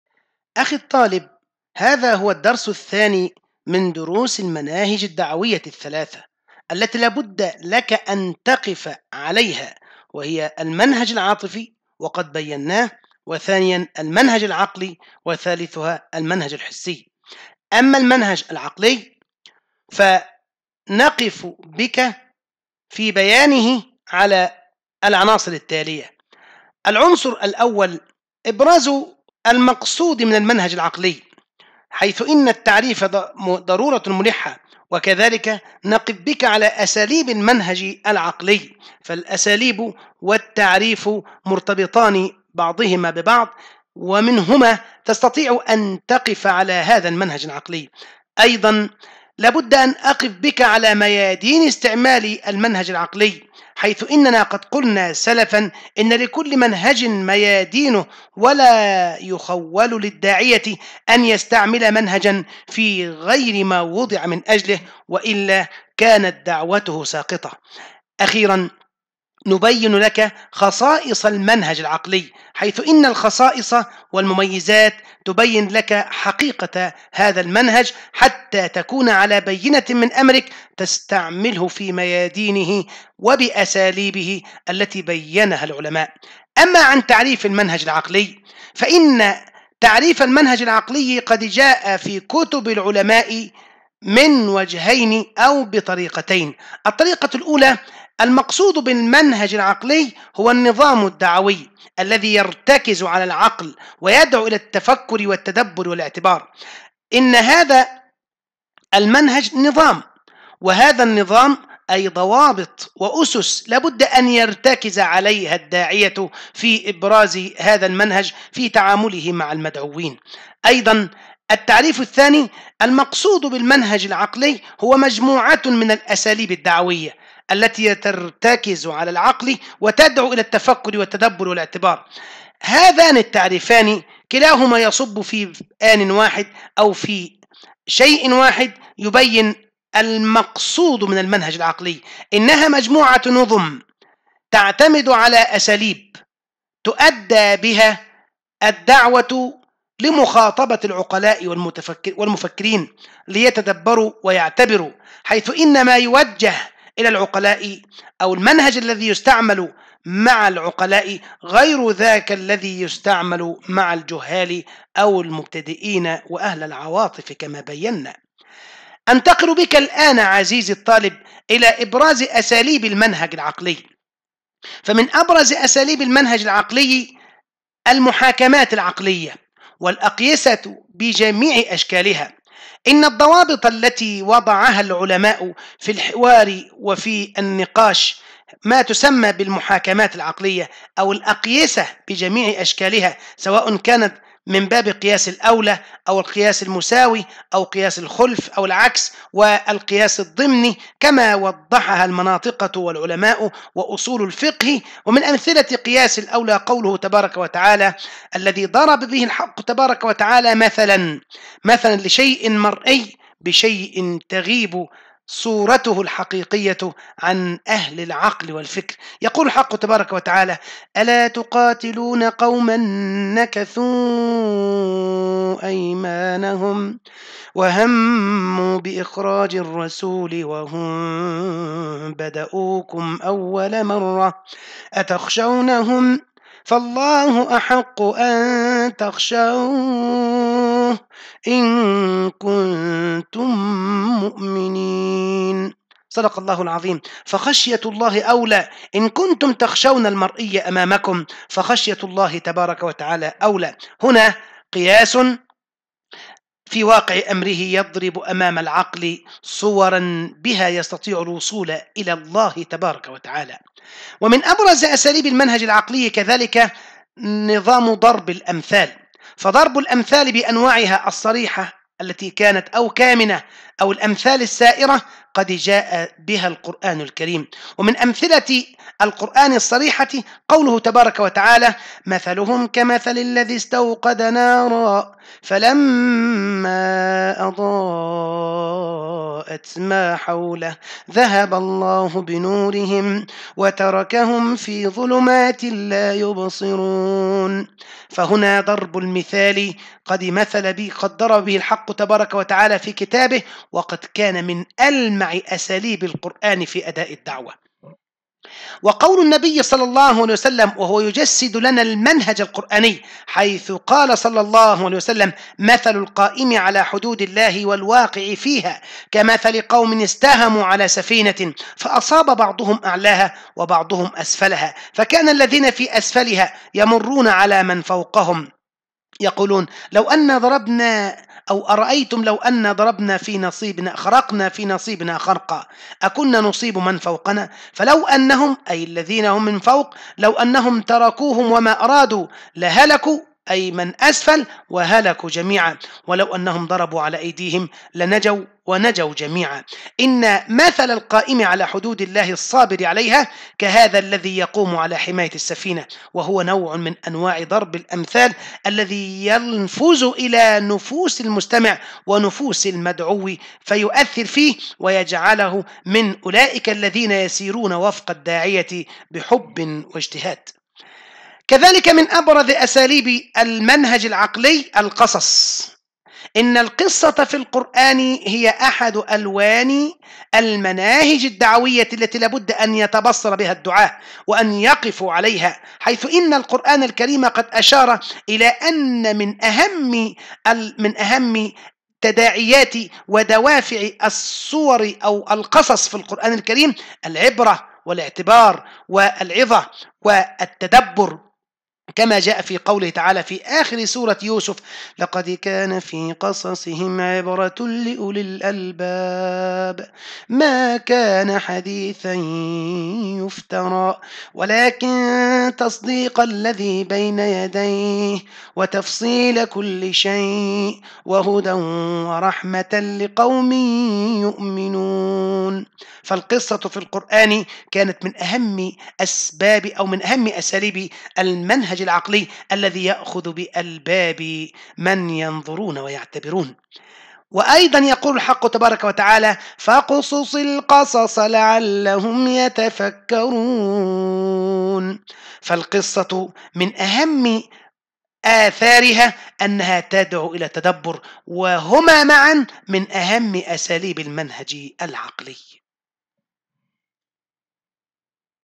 أصول دعوة 2 | الوحدة 2 | المنهج العقلي مفهومه أساليبه - 3
المقررات المفتوحة - أصول دعوة - 2 - 162 عقد
الوحدة 2 : المناهج الدعوية -أبرز أساليبها –مواطن استعمالها –خصائصها
الدرس 3 : المنهج العقلي مفهومه أساليبه - 3
-تعريف المنهج العقلي .
أبرز أساليب المنهج العقلي :
– المحاكات العقلية .
– ضرب الأمثال بأنواعها  .
-القصص التي يغلب عليها الجانب العقلي .